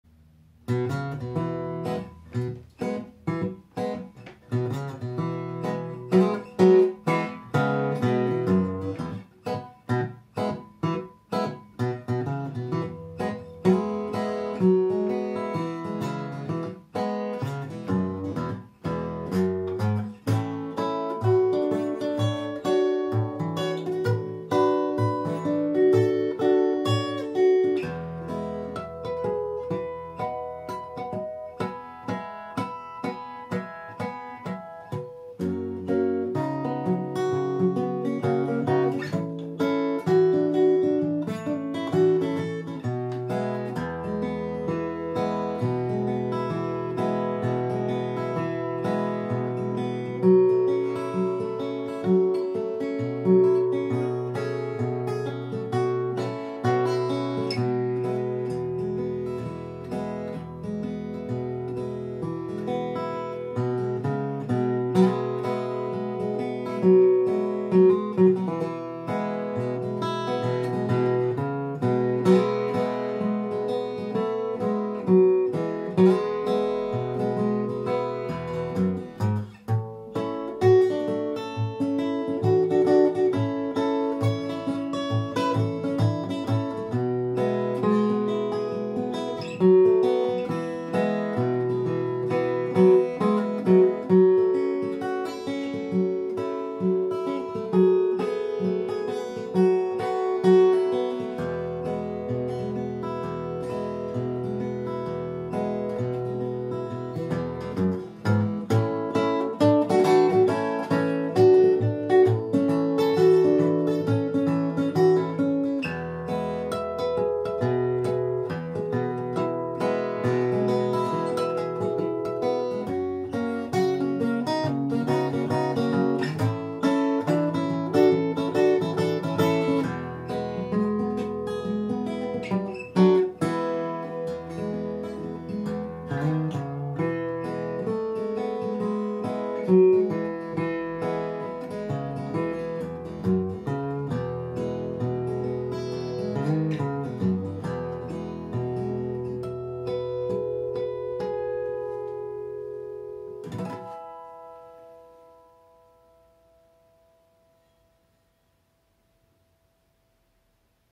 Ensaio #5 Del Vecchio 3/4
Afinação alternativa no Del Vecchio traveler desenvolvendo um
tema inspirado na música folk americana em ritmo de Fox.

Created by Wafer Music Art